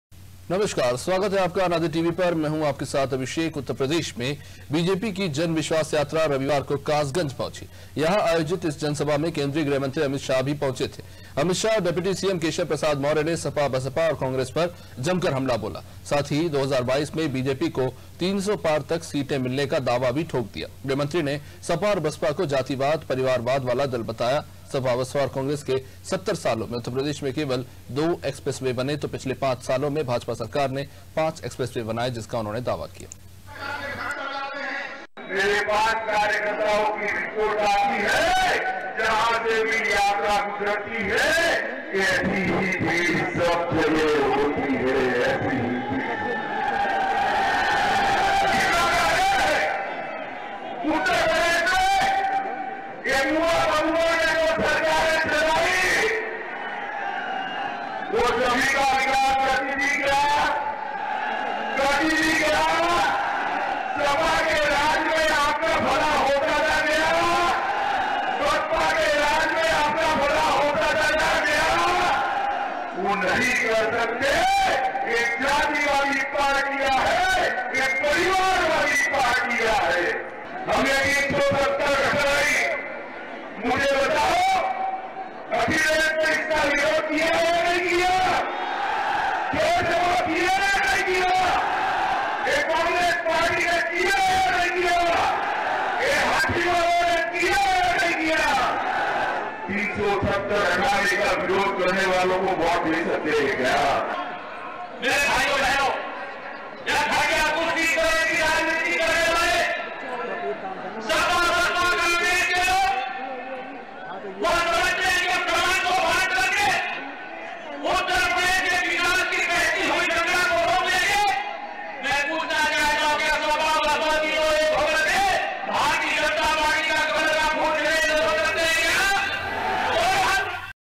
Kasganj : जनविश्वास यात्रा में बोले अमित शाह | UP News | Hindi news | Latest news | Anaadi TV
उत्तरप्रदेश में बीजेपी की जनविश्वास यात्रा रविवार को कासगंज पहुंची...यहां आयोजित जनसभा में केंद्रीय गृहमंत्री अमित शाह ने भी शिरकत की... अमित शाह और डिप्टी सीएम केशव प्रसाद मौर्य ने सपा-बसपा और कांग्रेस पर जमकर हमला बोला...साथ ही 2022 में बीजेपी को तीन सौ पार सीटें मिलने का दावा भी किया...केंद्रीय गृहमंत्री ने सपा-बसपा को जातिवादी और परिवारवादी दल बताया...सपा, बसपा और कांग्रेस के 70 सालों में उत्तर प्रदेश में केवल दो एक्सप्रेस-वे बने... वहीं पिछले 5 वर्षों में भाजपा सरकार में प्रदेश में पांच नए एक्सप्रेस-वे बने हैं. ।